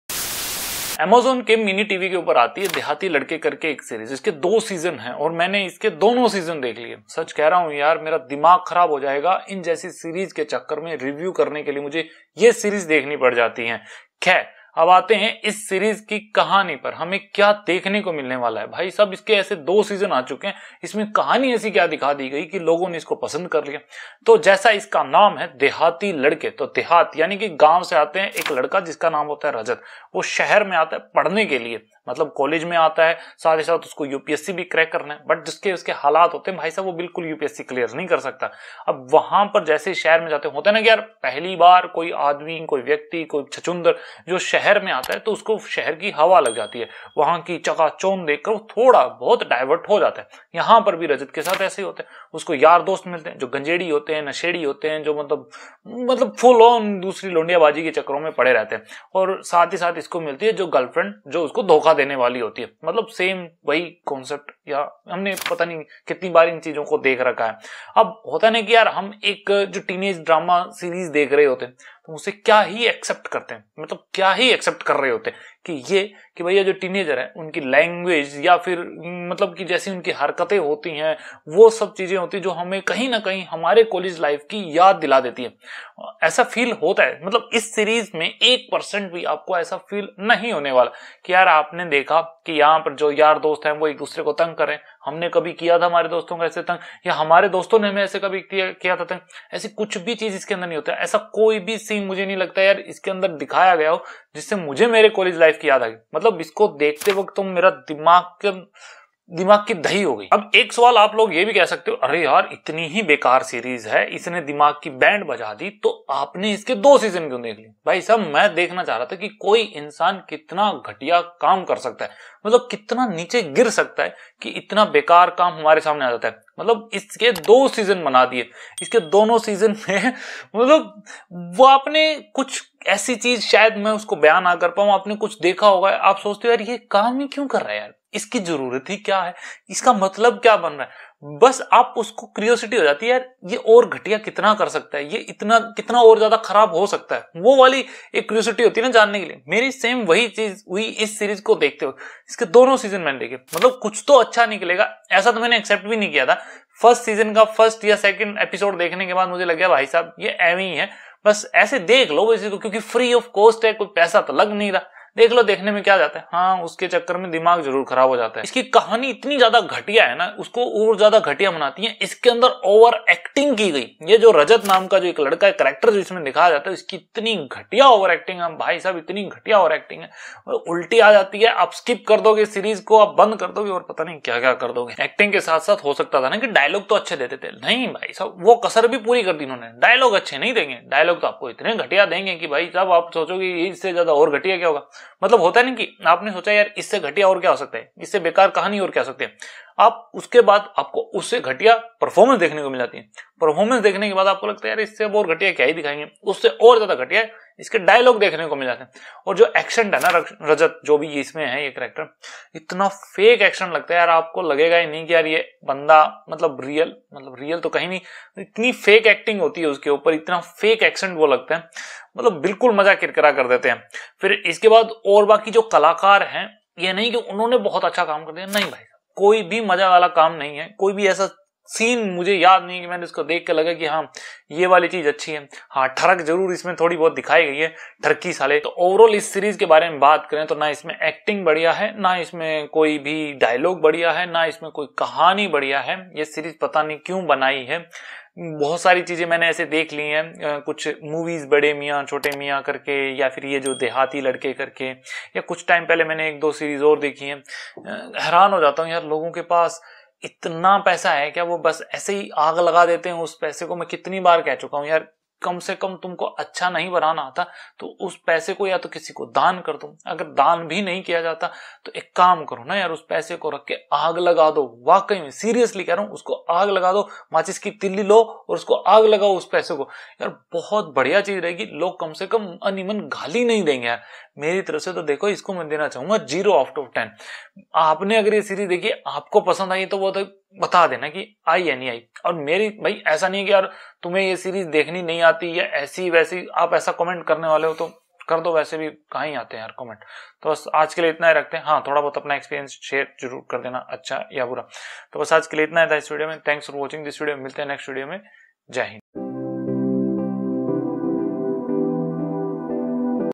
Dehati Ladke (2023) Web Series Review | Kusha Kapila, Aasif Khan, Saamya Jainn
An engrossing tale of Rajat's (Shine Pandey) journey of self-discovery as he moves from a village to a big city to pursue his dream of clearing the UPSC exams. The story follows Rajat as he explores his new-found freedom, builds new friendships, and experiences first love-all while navigating family pressures and staying true to his goal
रजत (शाइन पांडे) की आत्म-खोज की यात्रा की एक मनोरंजक कहानी, जब वह यूपीएससी परीक्षा पास करने के अपने सपने को पूरा करने के लिए एक गांव से एक बड़े शहर में जाता है। कहानी रजत की है जो अपनी नई-नई आज़ादी की खोज करता है, नई दोस्ती बनाता है, और पारिवारिक दबावों को झेलते हुए और अपने लक्ष्य के प्रति सच्चे रहते हुए पहले प्यार का अनुभव करता है।